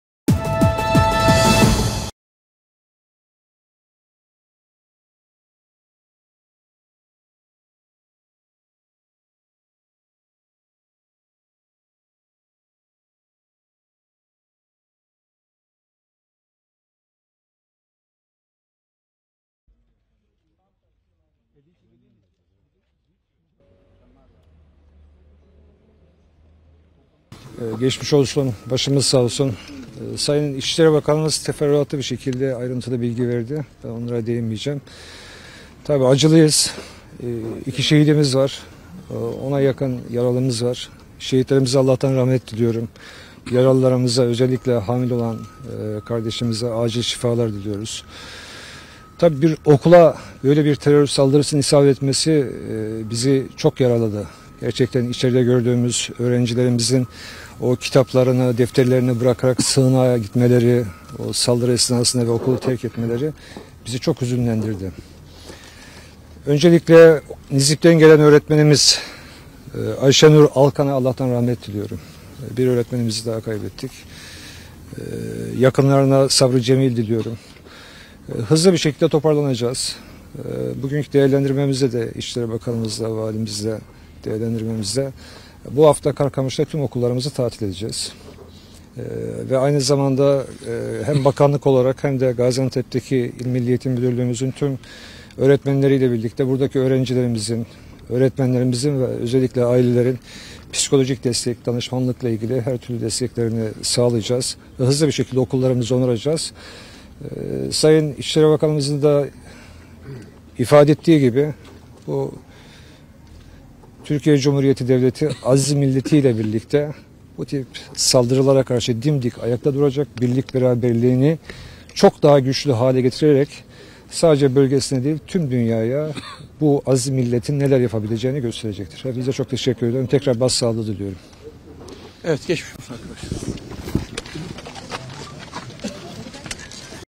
Mahmut Özer Bu Hafta Karkamış'ta Bütün Okulları Tatil Edeceğiz
Milli Eğitim Bakanı Mahmut Özer: "Bu hafta Karkamış'ta bütün okulları tatil edeceğiz."